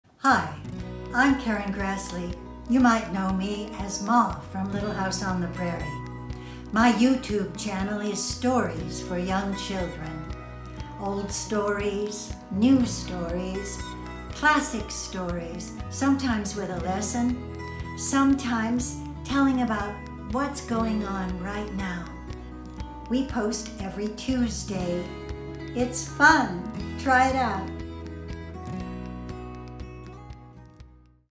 Karen Grassle's Channel Intro
Welcome to Karen Grassle's YouTube channel, where Karen reads stories for young children--some old, some new! We post a new story each week on Tuesdays.

Karen Grassle is best known for her portrayal of Ma on Little House on the Prairie and her work with women's issues.

Follow Karen on Facebook for news and updates.